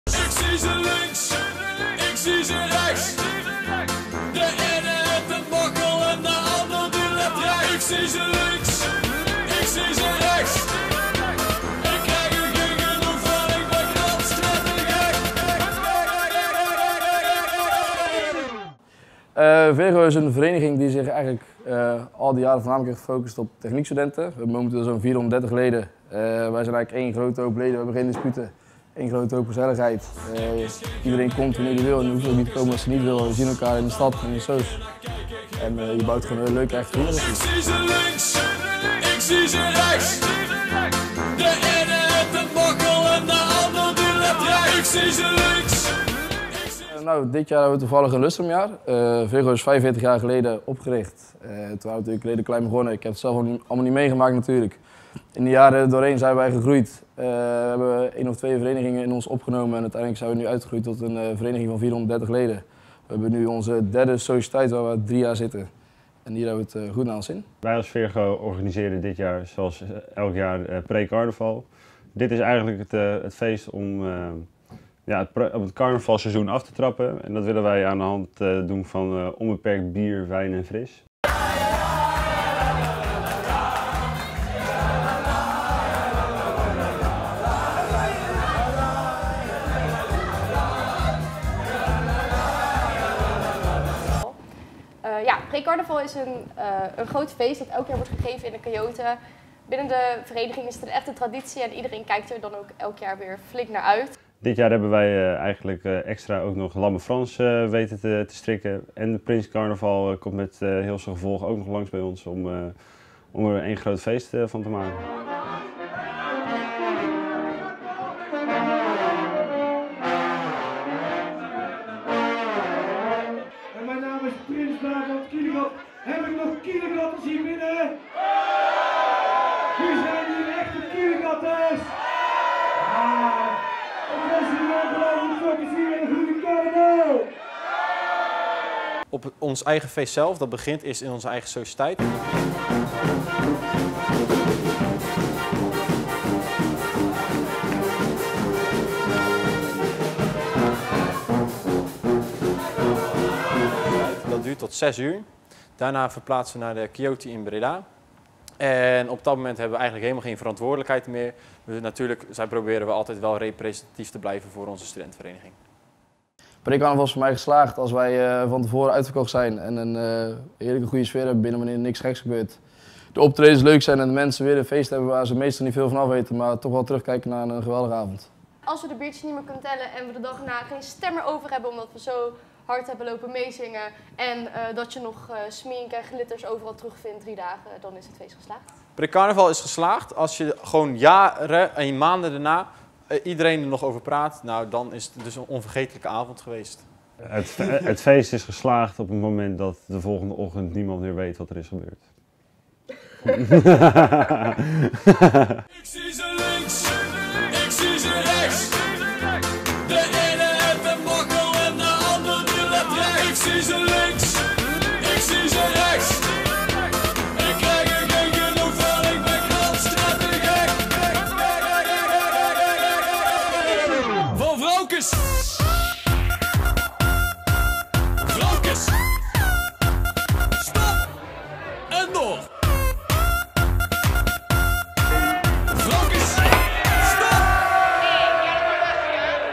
Pré-Carnaval SV Virgo
Onze razende student-reporter ging langs bij Studentenvereniging Virgo Breda. Hun Pré-Carnavalsfeest is een echt begrip. Maar komt dat dan?